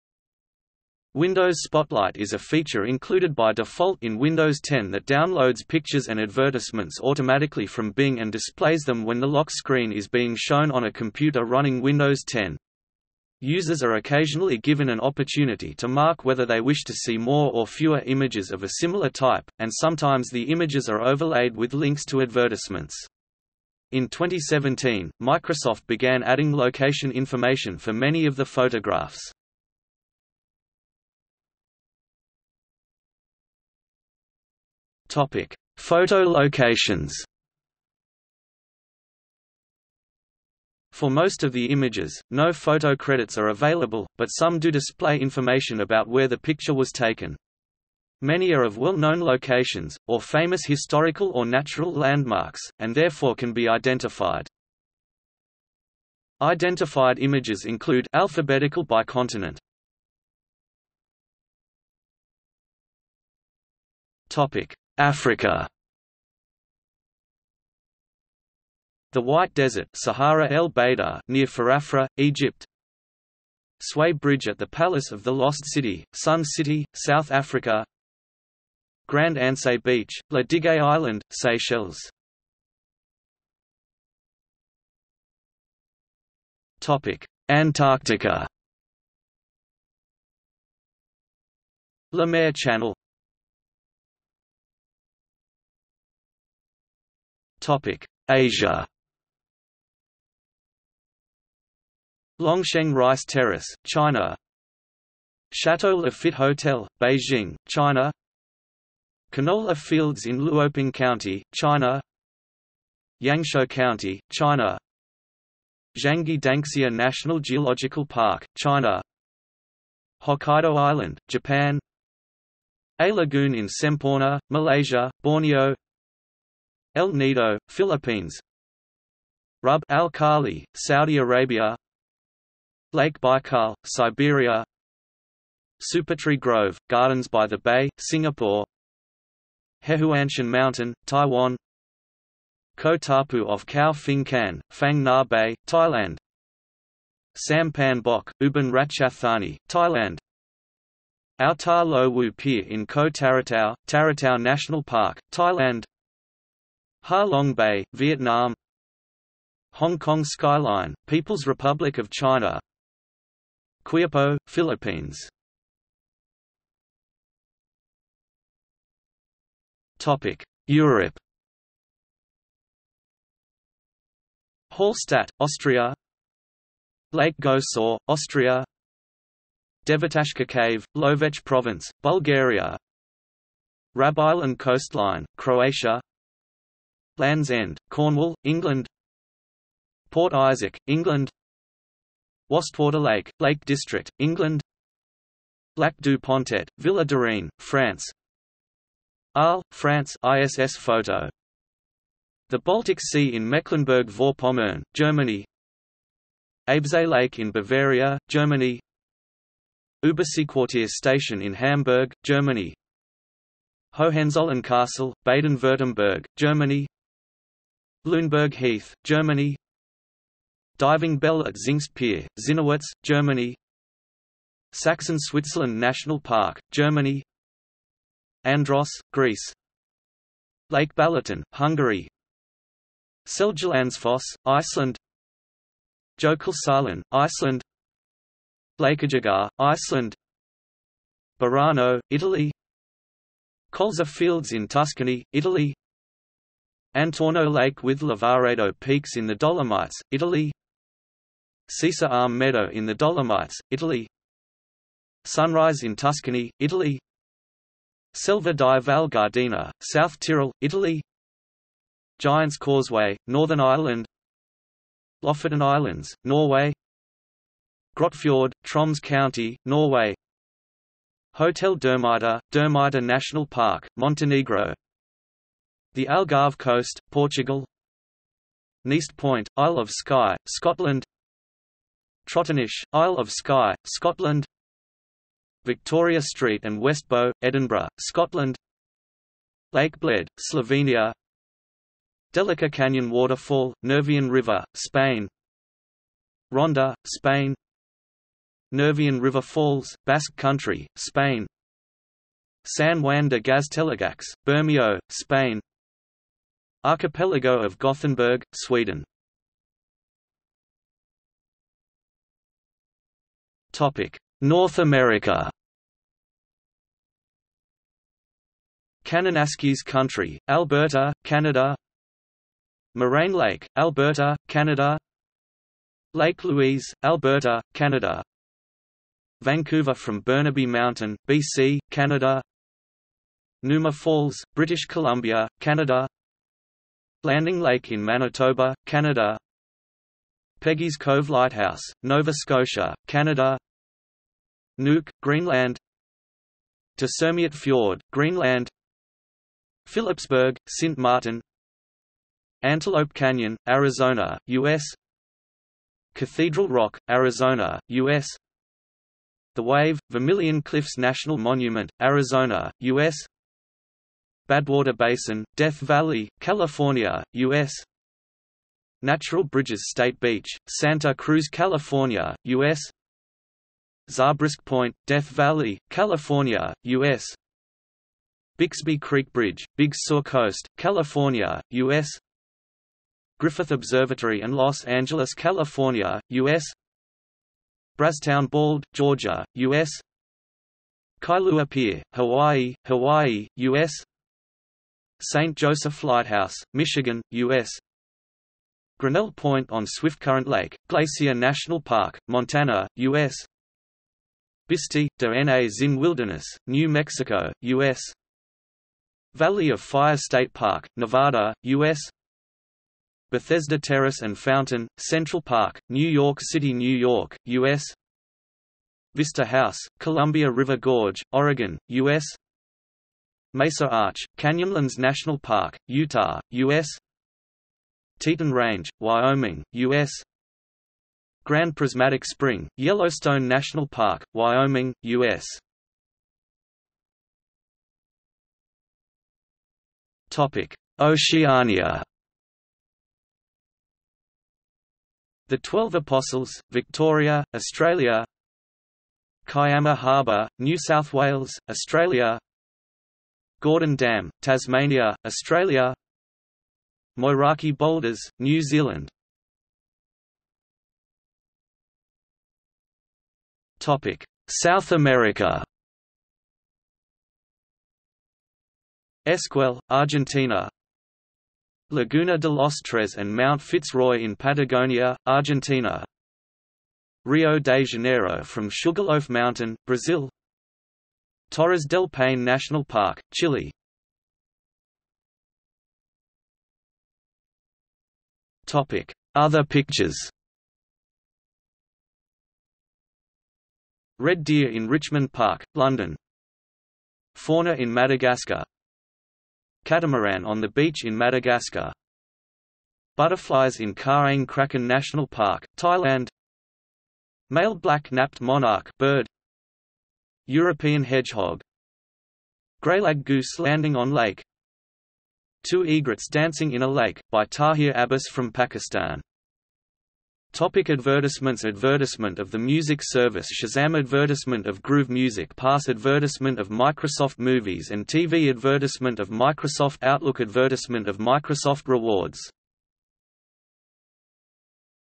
Windows Spotlight | Wikipedia audio article
00:00:31 1 Photo locations
00:01:00 1.1 Africa
00:01:23 1.2 Antarctica
00:01:34 1.3 Asia
00:02:46 1.4 Europe
00:05:10 1.5 North America
00:07:16 1.6 Oceania
00:07:40 1.7 South America
00:08:04 2 Other pictures
00:08:40 3 Advertisements

- Socrates

SUMMARY
Windows Spotlight is a feature included by default in Windows 10 that downloads pictures and advertisements automatically from Bing and displays them when the lock screen is being shown on a computer running Windows 10. Users are occasionally given an opportunity to mark whether they wish to see more or fewer images of a similar type, and sometimes the images are overlaid with links to advertisements. In 2017, Microsoft began adding location information for many of the photographs.